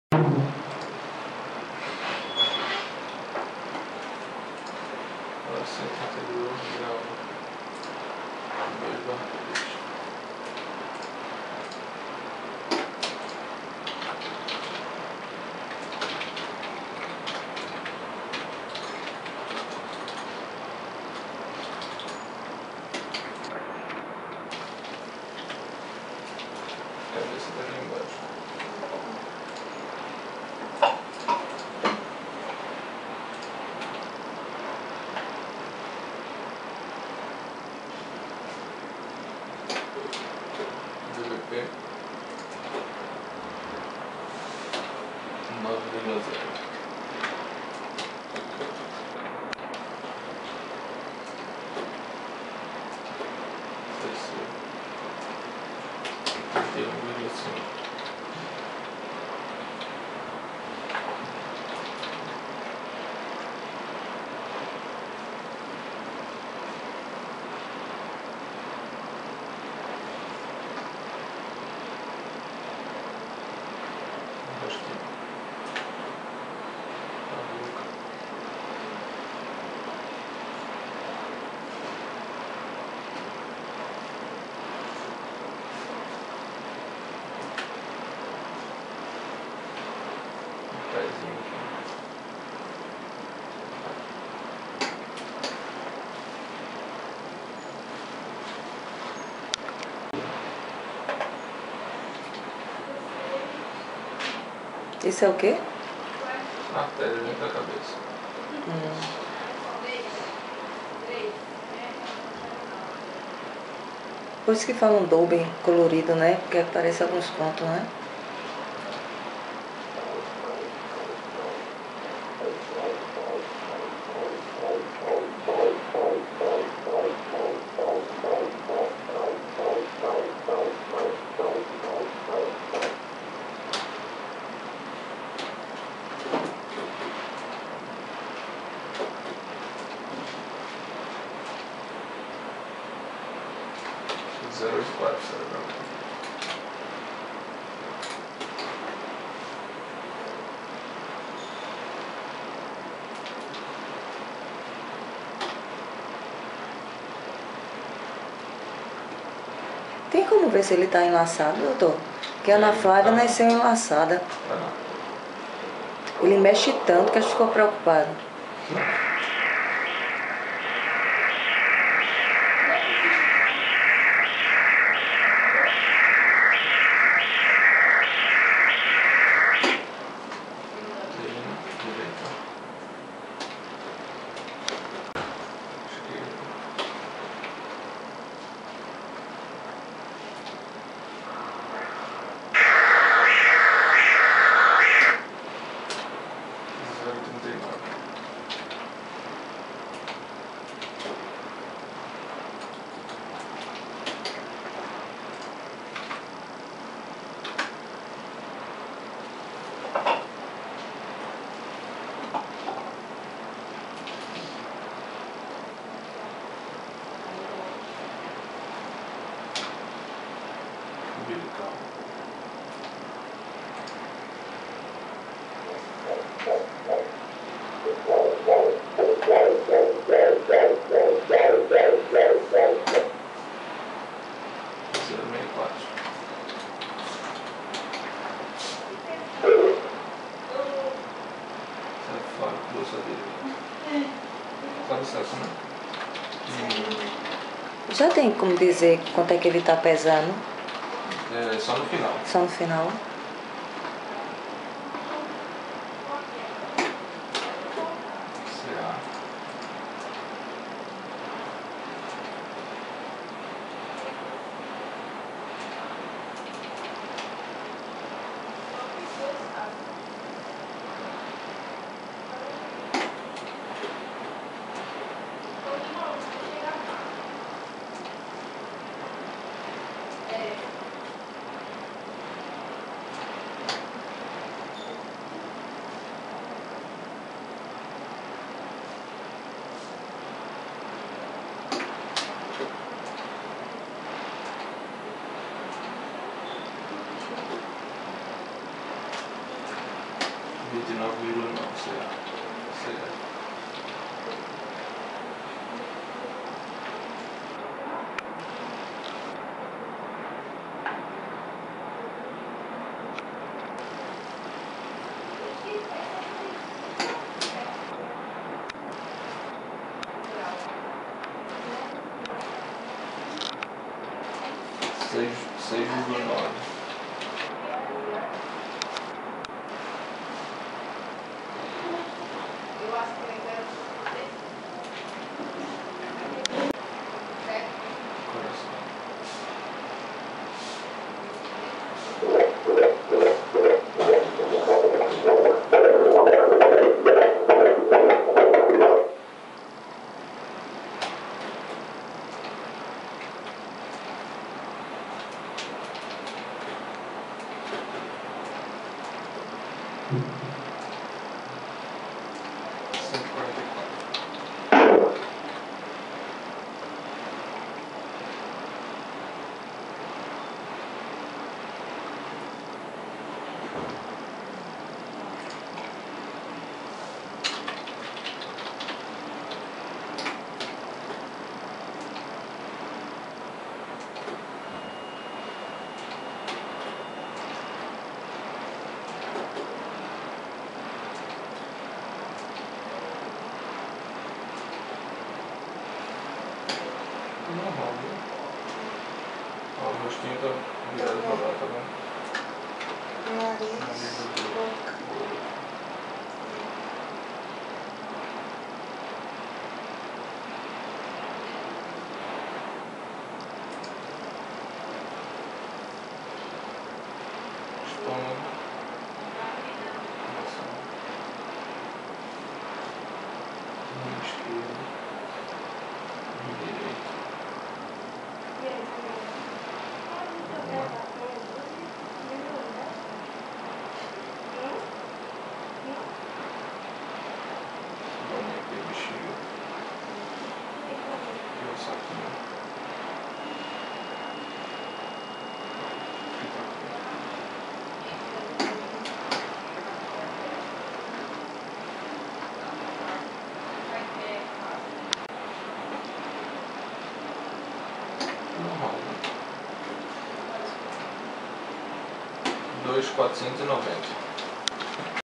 Ultra com Doppler Colorido - Gabriel Ebrahim - 35 semanas
35 semanas da minha vida, dentro da minha vida (: GABRIEL EBRAHIM ♥
8 de março de 2012

Quem assistiu a ultra? Vovó Rossandra e Vovô Flávio